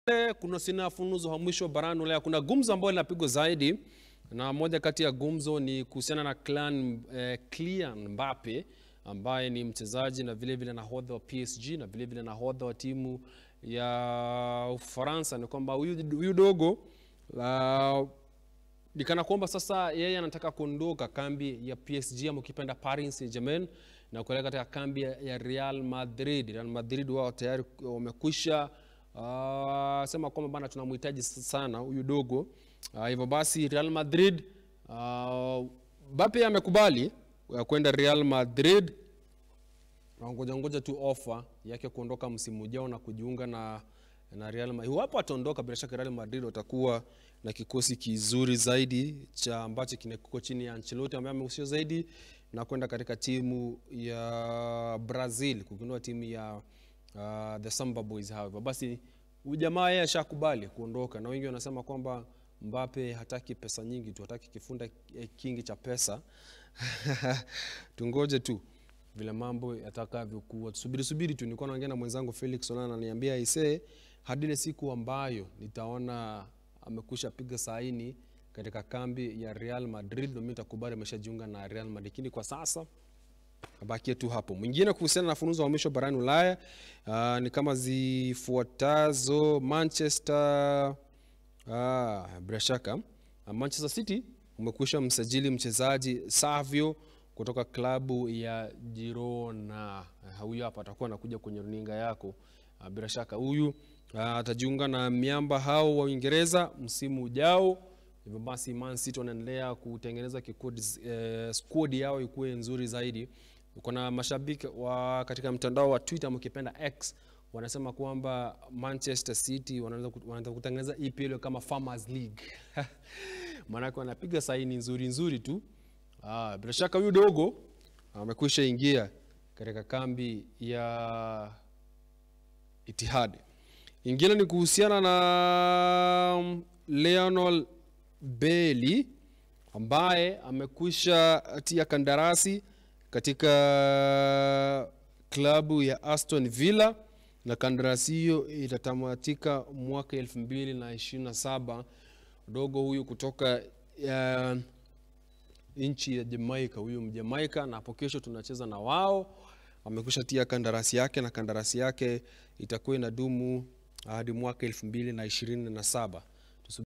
Manchester City Wamemsajili Mchezaji Savio Kutoka Klabu Ya Girona
Fununu Za Uhamisho Barani Ulaya

Manchester City Wamemsajili Mchezaji Savio Kutoka Klabu Ya Girona, Savior Atajiunga Na Miamba Hao Wa Uingereza Msimu Ujao.